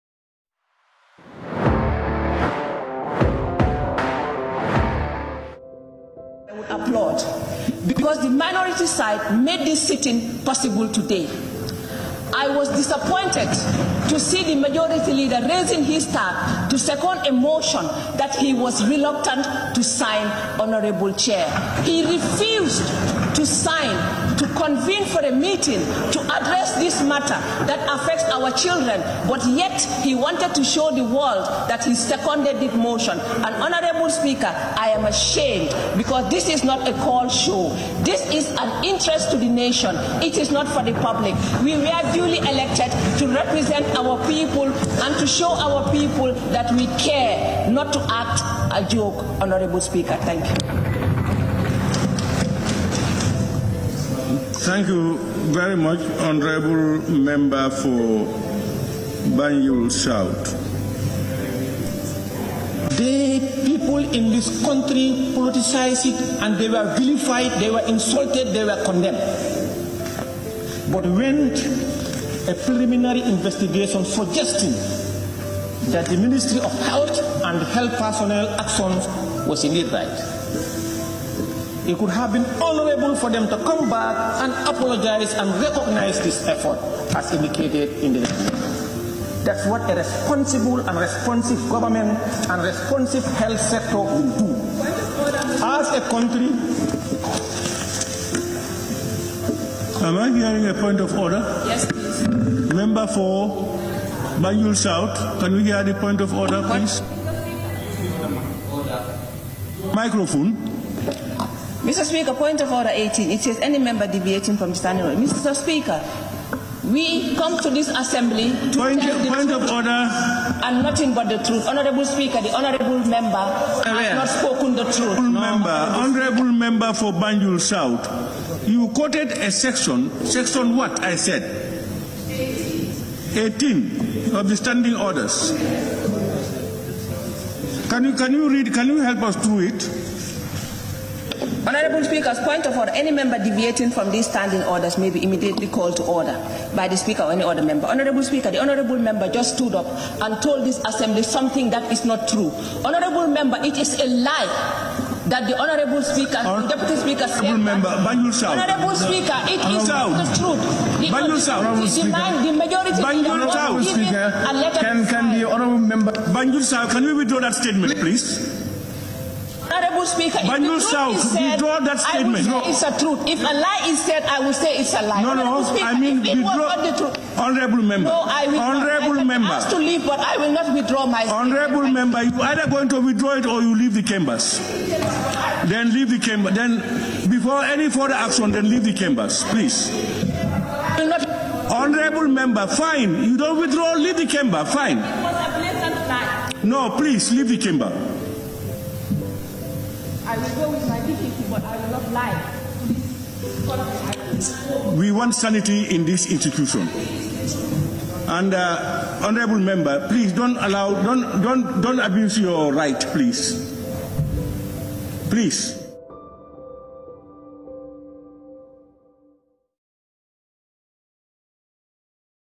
Our NAM's on flames🔥Touma termed Seedy Njie as a liar😱FTJ Concludes
Our Parliament was on fire
Hon Touma Njie angrily opposes the deputy speakers opinions & Fabakary Tombong Jatta ask her to live the premises.
THE JOY OF FREEDOM OF SPEECH🇬🇲